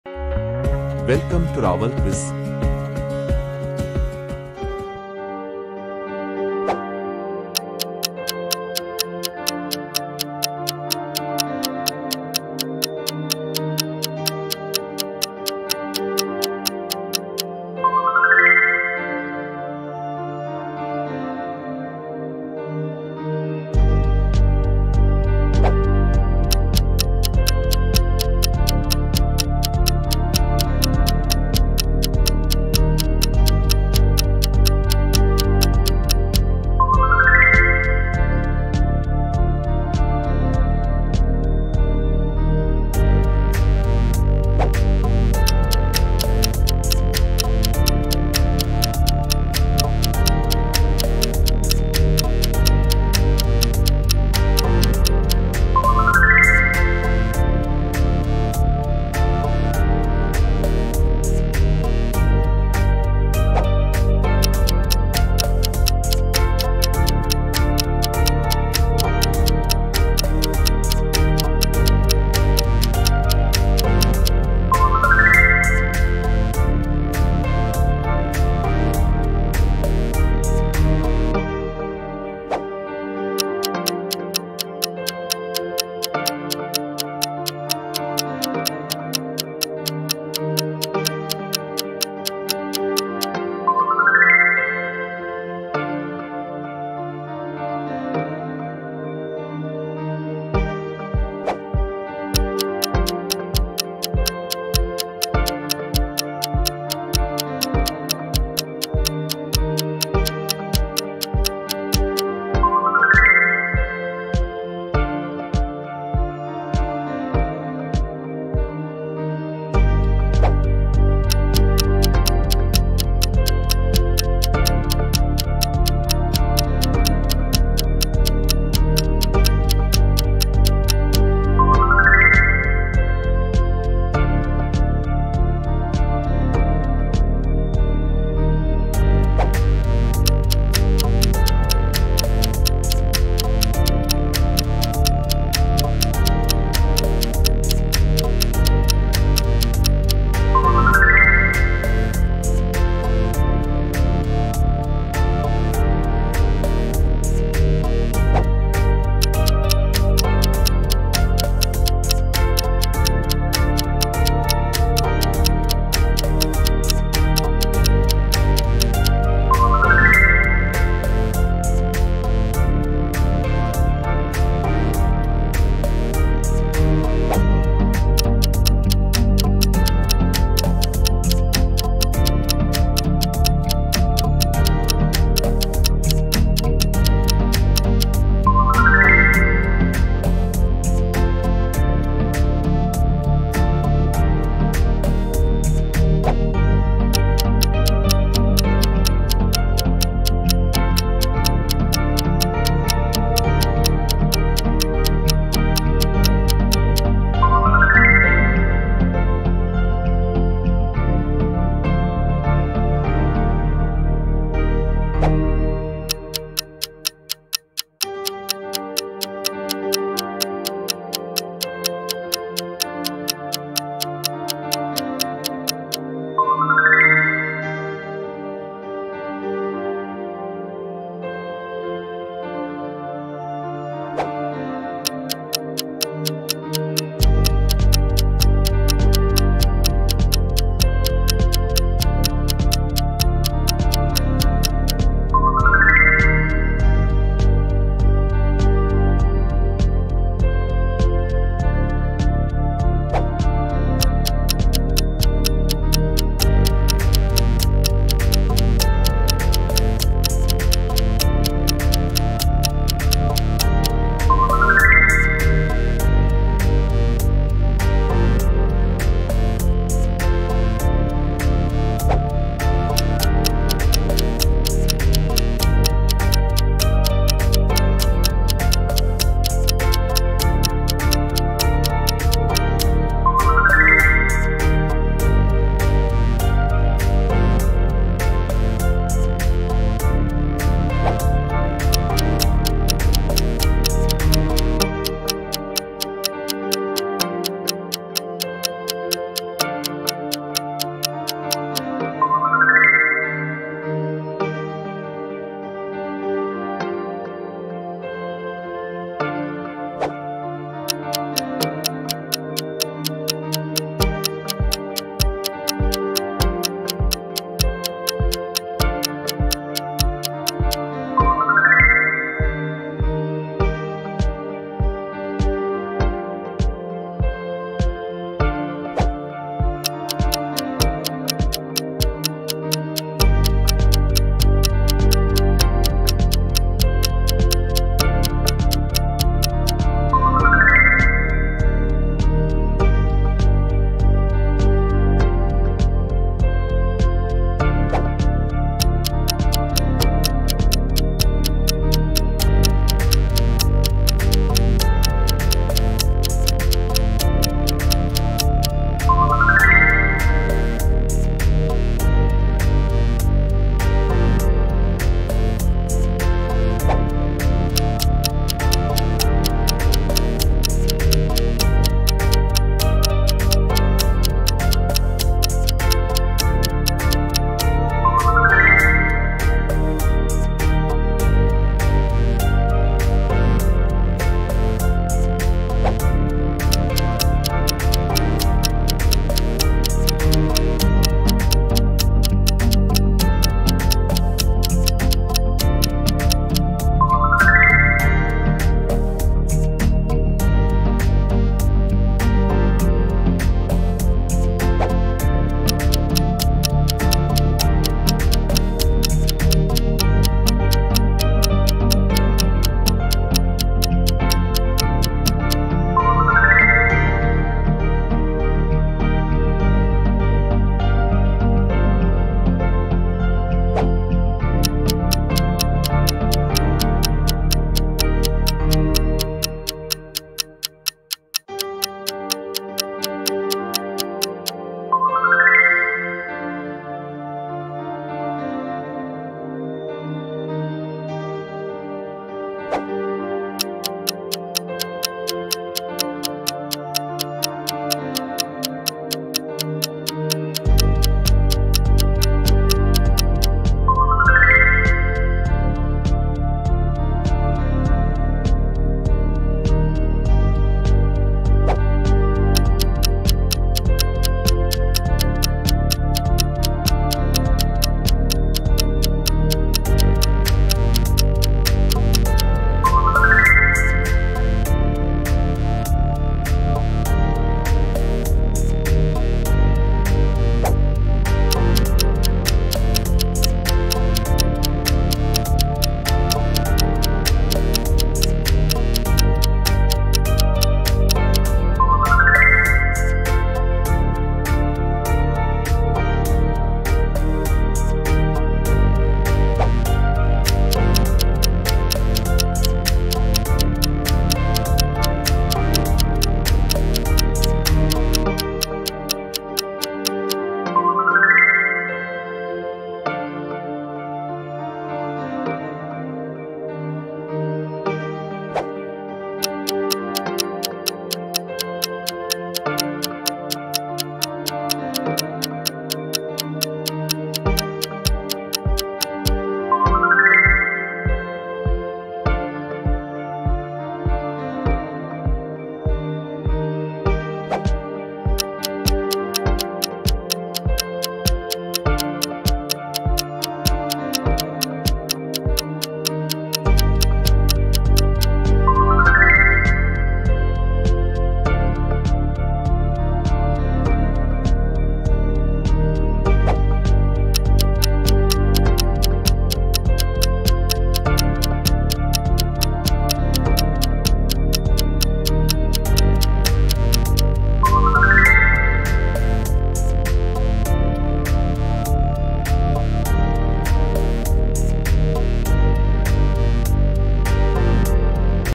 Guess The Country by The National Animal || Country Quiz Challenge || Rawal Quiz
Can you guess the country by its national animal?

This country quiz will test your knowledge about countries national animals.
There are also amazing facts about these animals.
Here are such countries as Australia, India, Russia, United Kingdom, Sweden, Brazil, China, Thailand and others.
👍Hope you like this geography quiz game :)